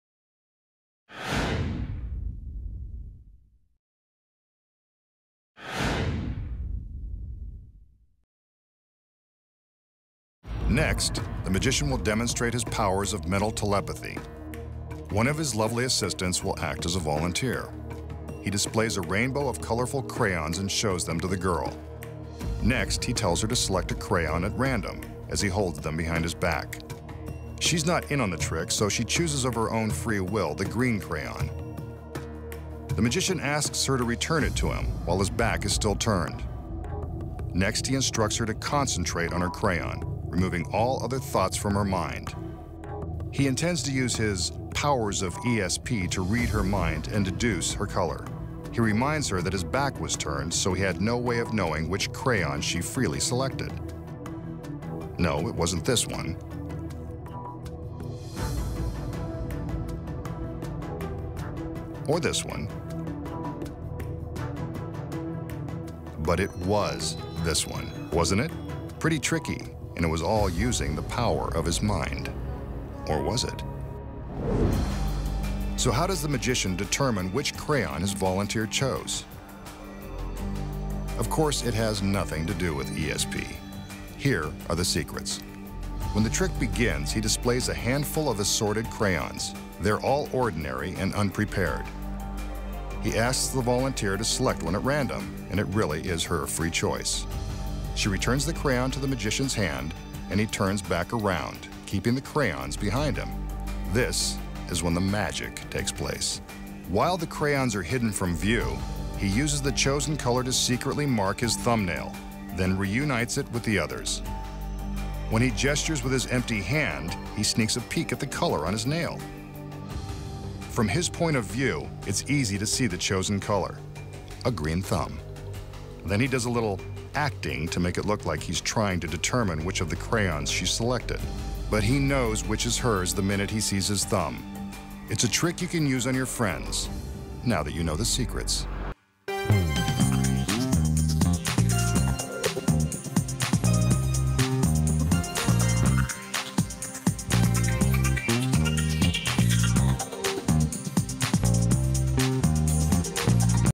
TRICK YOUR FRIENDS WITH YOUR POWER OF PERCEPTION!
A classic trick that's sure to have your friends scratching their heads! A volunteer picks a crayon at random from the Masked Magician while his back is turned. How does he know which color she picked? Watch to uncover this simple secret!